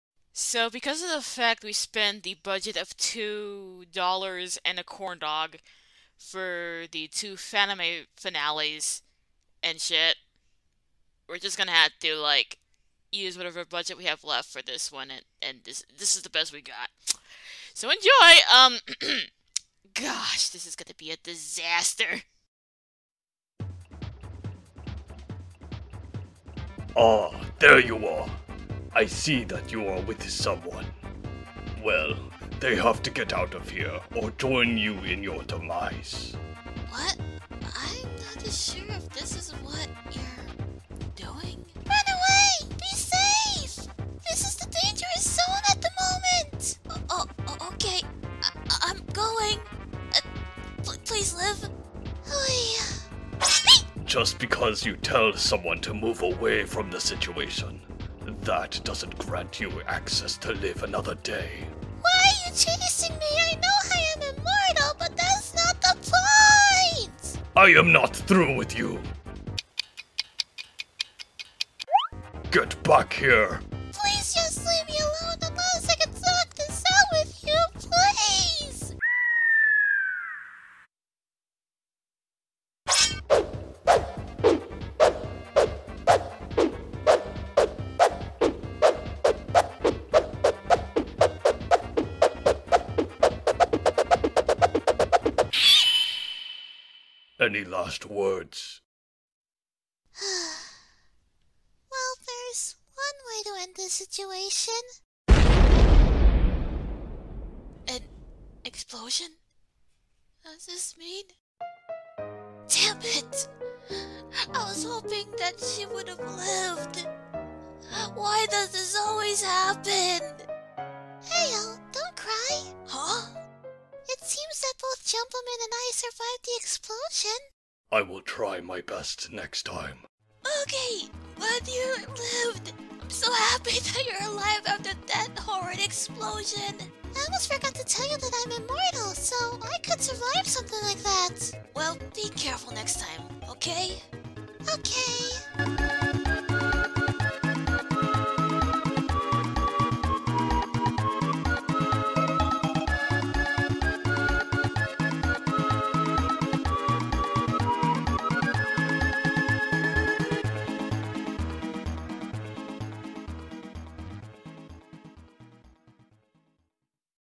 The End of the Mugi Baloogie Saga (Gissy is Bored Challenge)
Do ya'll have moments when you are so bloody bored that you just wanted to have fun and make a shitpost?

Because that's what happened. I've made the finale of the Mugi Baloogie Saga because I was so bored, LMAO.

But yeah, I've made the finale for a fanime that few people asked for. ^^;

Also, just to be safe since it's more proper this way, Elizabeth belongs to Doodletones.

Also, Happy Easter